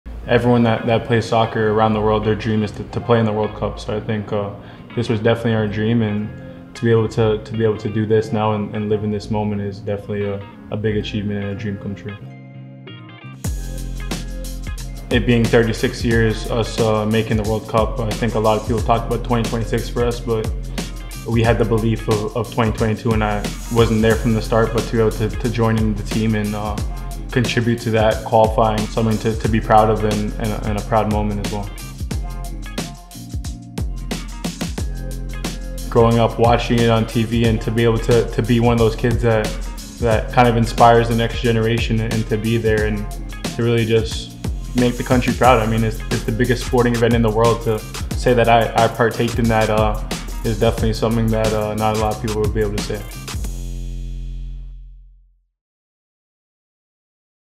Dayne St. Clair on his World Cup Experience
Minnesota United FC and Canada goalkeeper Dayne St. Clair on representing his country, inspiring the next generation and more.